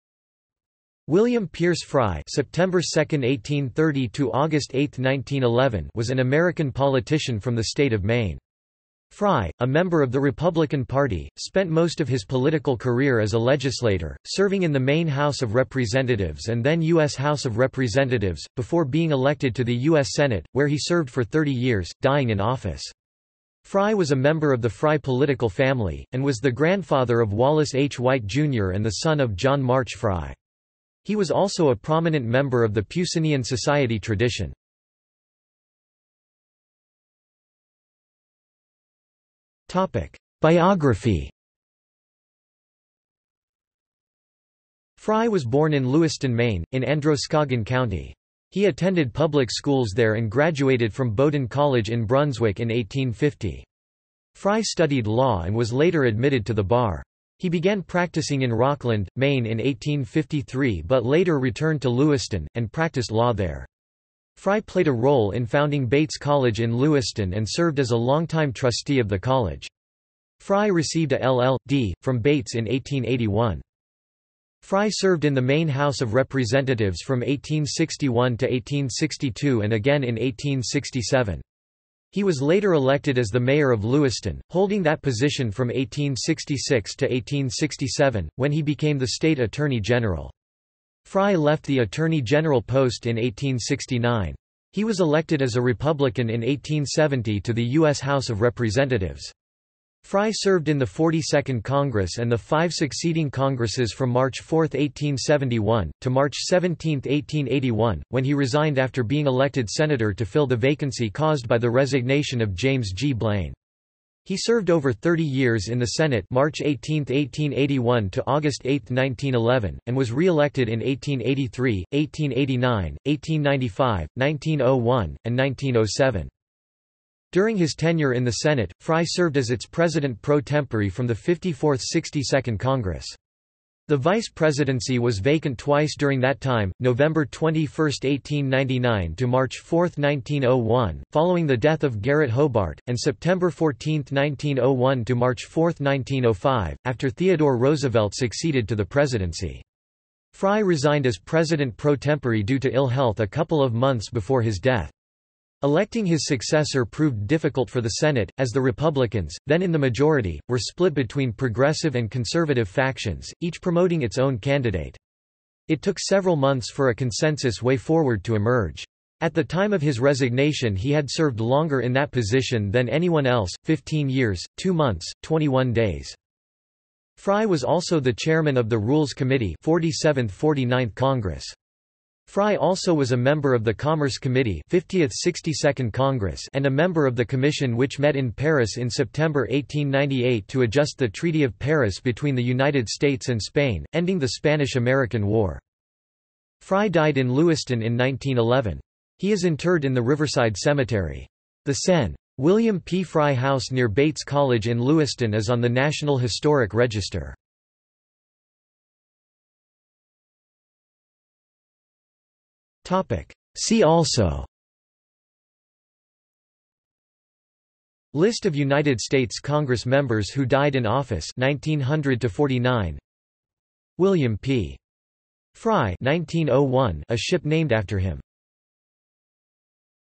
William P. Frye | Wikipedia audio article
This is an audio version of the Wikipedia Article:
William P. Frye

00:00:36 1 Biography
00:03:29 2 See also

- Socrates

SUMMARY
William Pierce Frye (September 2, 1830 – August 8, 1911) was an American politician from the state of Maine. Frye, a member of the Republican Party, spent most of his political career as a legislator, serving in the Maine House of Representatives and then U.S. House of Representatives, before being elected to the U.S. Senate, where he served for 30 years; dying in office. Frye was a member of the Frye political family, and was the grandfather of Wallace H. White, Jr. and the son of John March Frye. He was also a prominent member of the Peucinian Society tradition.